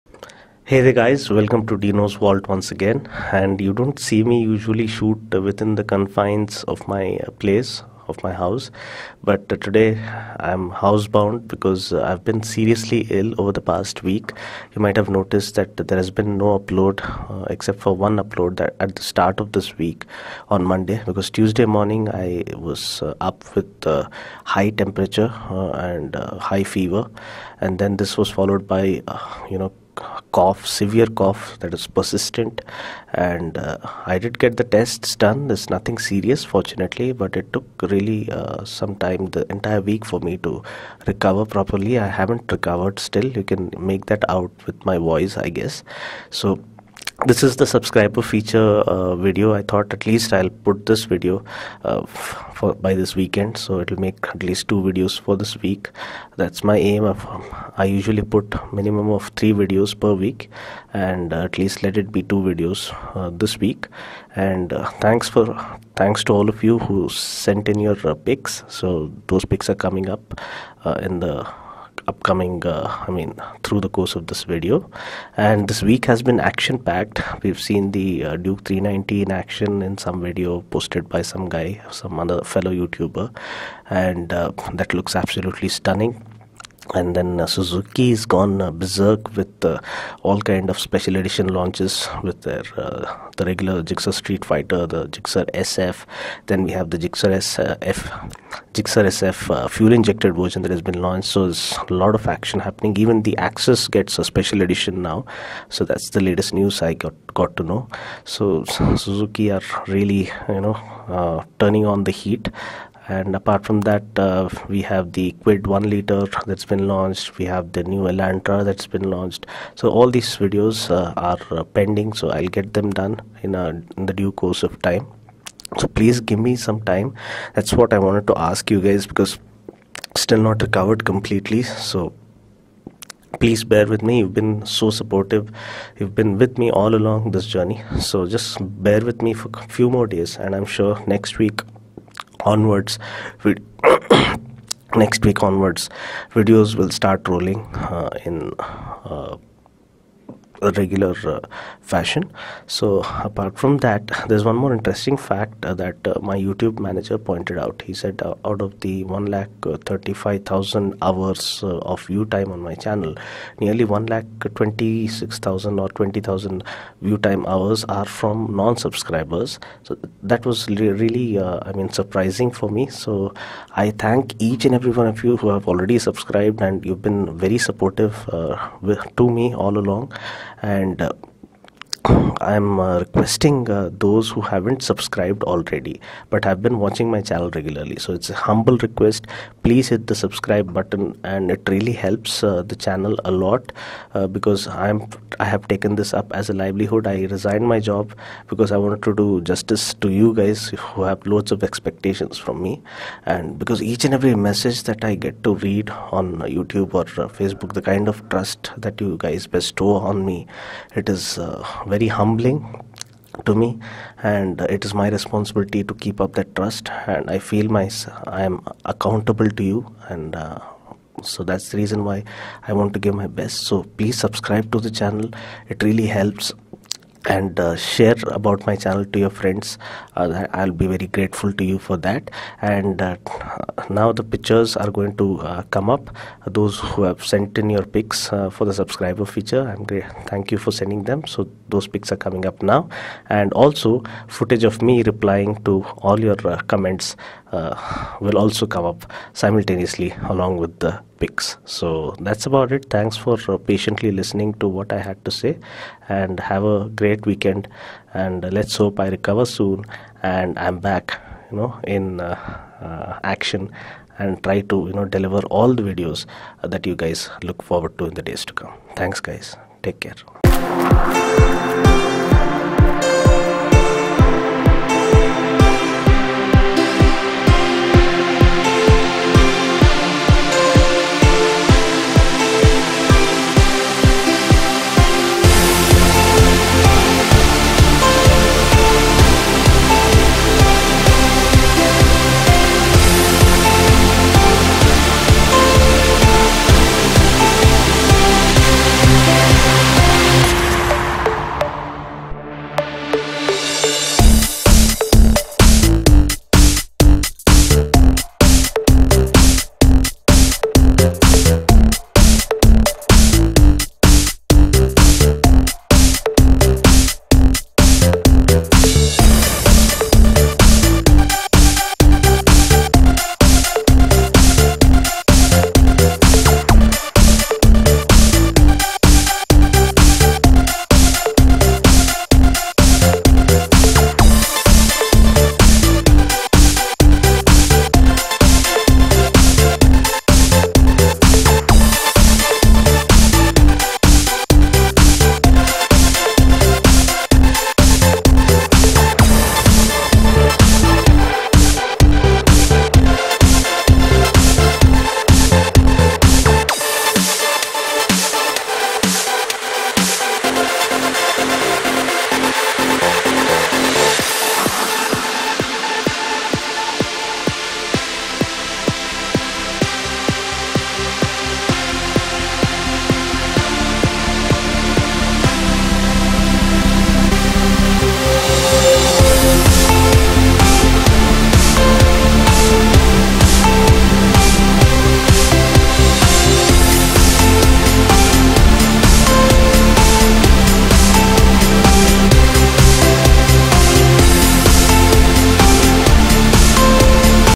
DinosSubscriberFeature: Ep. 02, Health Update
Sorry guys, I was down with very high fever and severe persistent cough. I don't drink, nor do I smoke and the cough was too scary because it was very intense. Thankfully by God's grace after getting all the tests done, there's nothing serious. I am feeling better now and should start uploading videos next week onwards.
Thanks for all the love and trust all along. Those who want to get featured in next month's Subscriber Feature, please send in your pics via message